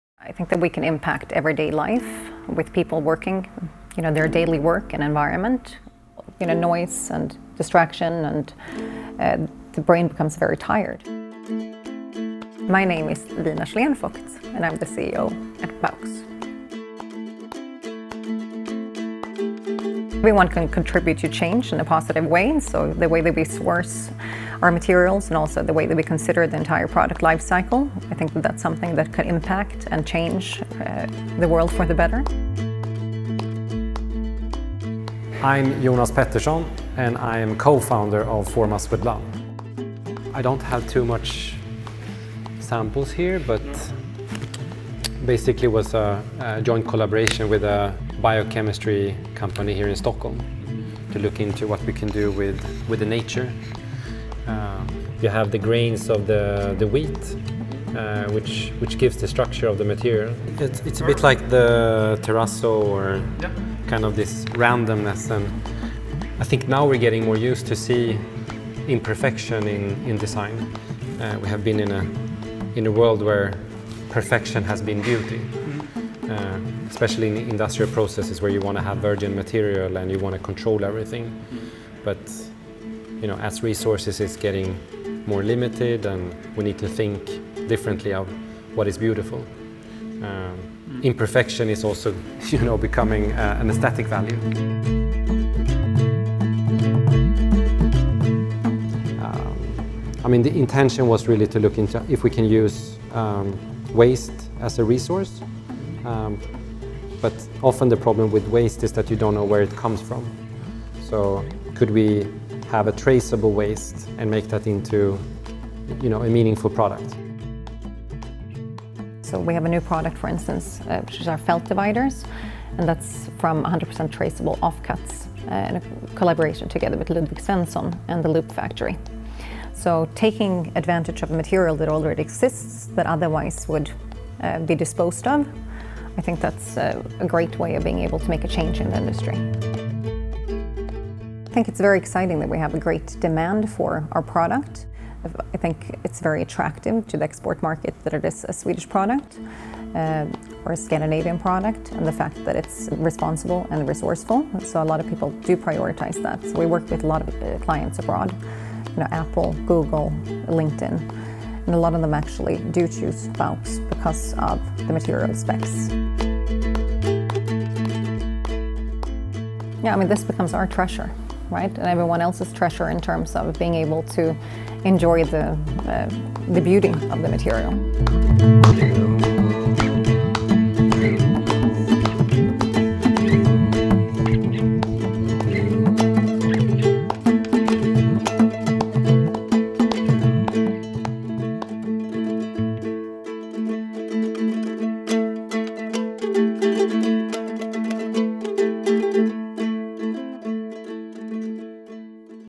Baux – on sustainable acoustics
Designers at Baux have worked to improve people’s lives at work and to pioneer sustainable materials in acoustics since 2013. Their noice-reducing construction materials are inspired by the colours, patterns and shapes in nature. Baux's latest solution for a healthier planet is felt made from upcycled and 100% traceable post-production textiles.

This film is part of a series made by the Swedish Institute and Svensk Form (the Swedish Society of Crafts and Design). Producer: Note (design studio); Director of Photography: Carl Engberg.